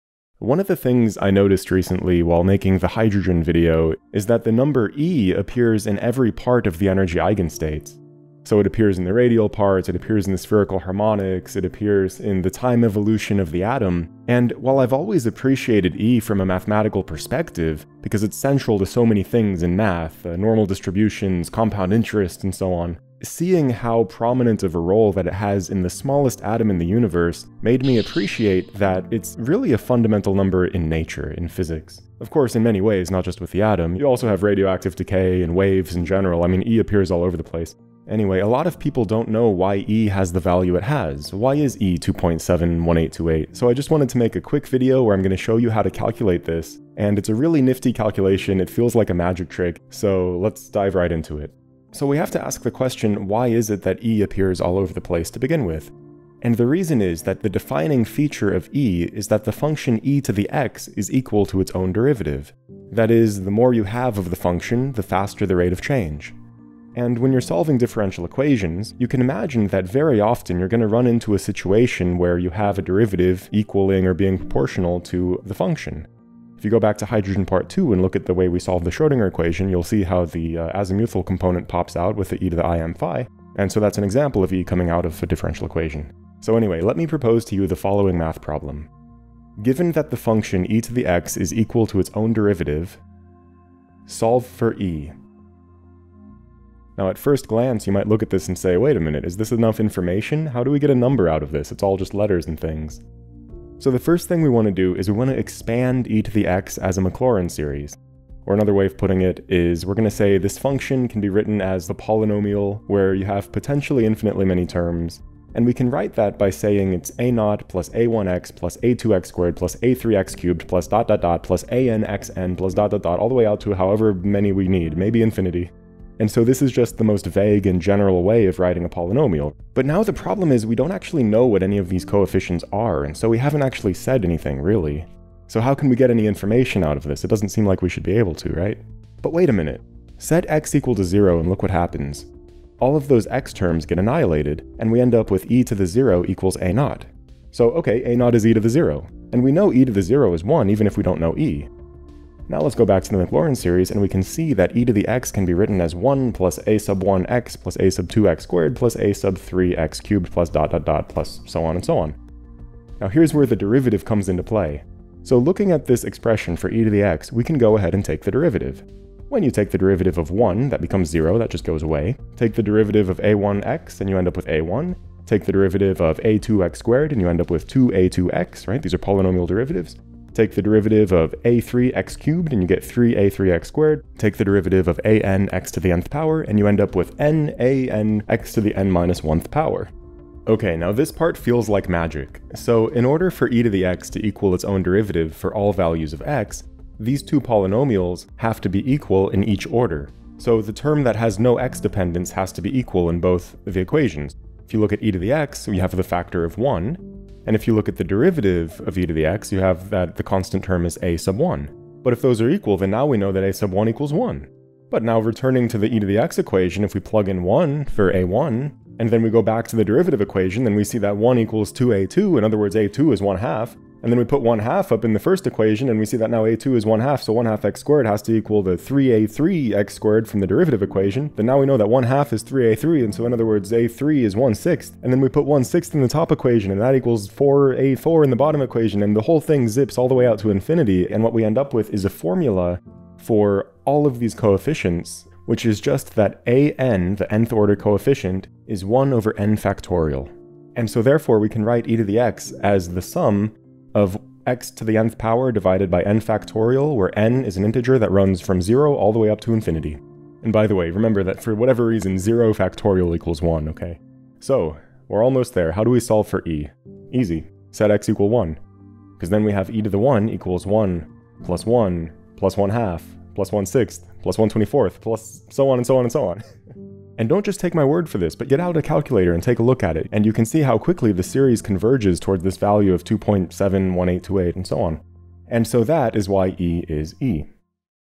Why e is e (Calculating Euler’s Number)
In this video, we explore why e (Euler’s number), which appears throughout math and science, in everything from the hydrogen atom, harmonic oscillator, radioactive decay, waves, to Gaussian distributions, compound interest, and all kinds of other things, has the value of 2.71828…

It’s a quick and memorably derivation, and I hope you enjoy! :)